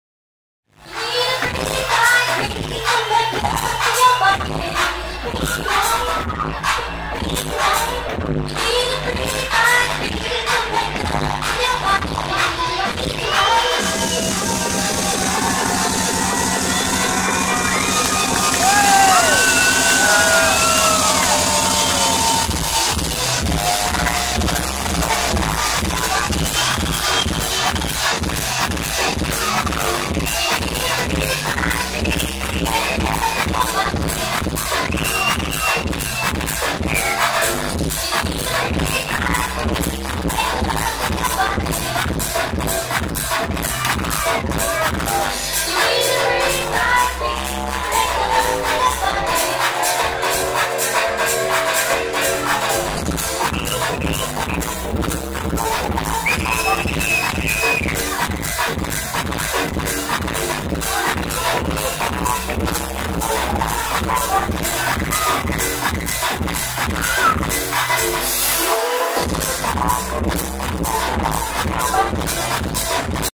Loco Dice Closing District 8
Loco Dice playing the very last track of the night at District 8, place was on wheels!